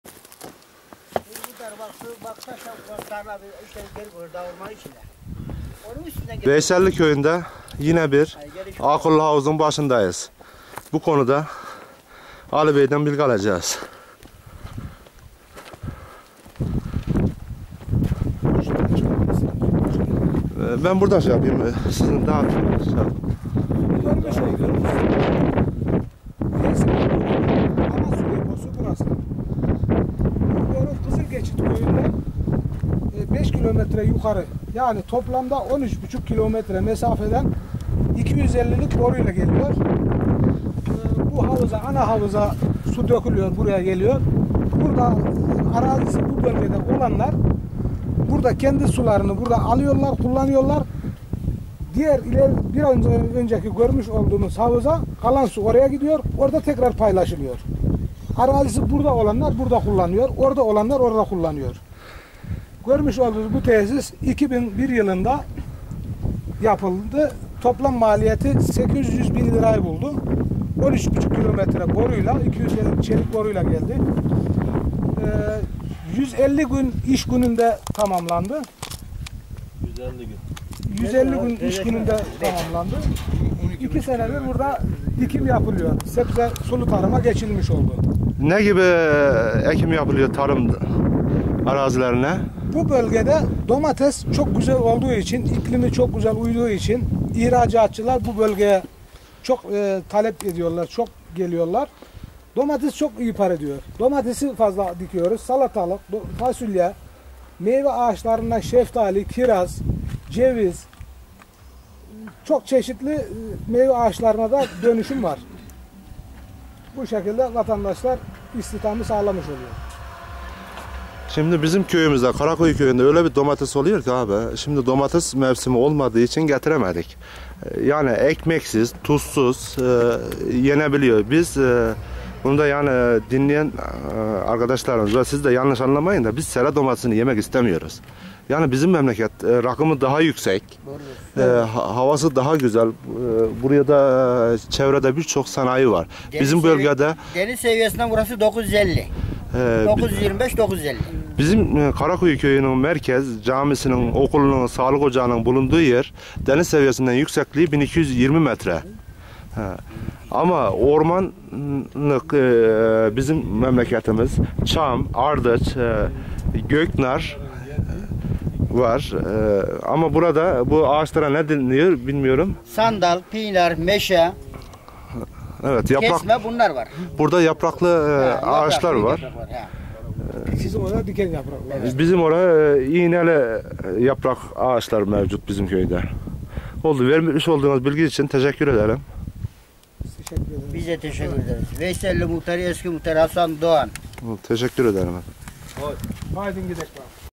HAK YEMEZ HAVUZ 2
Karakuyu Köyüne Tarım Sulama suyu nasıl cıkarılır diye Mersin Erdemli ilçesine bağlı köyleri gezdik ve bilgi aldık. Akıllı havuz 2